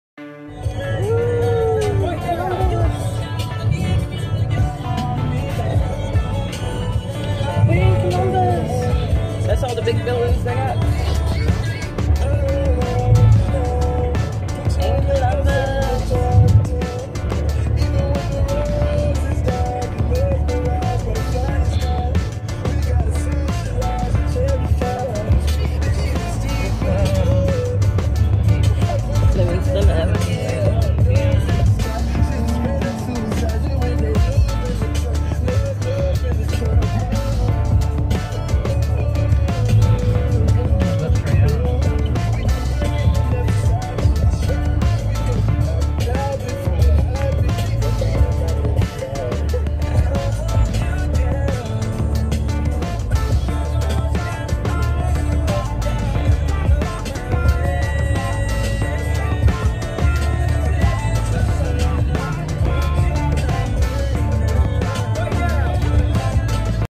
Morning routine! On my way to Columbus Ohio
Columbus Ohio it's so much fun never been to Columbus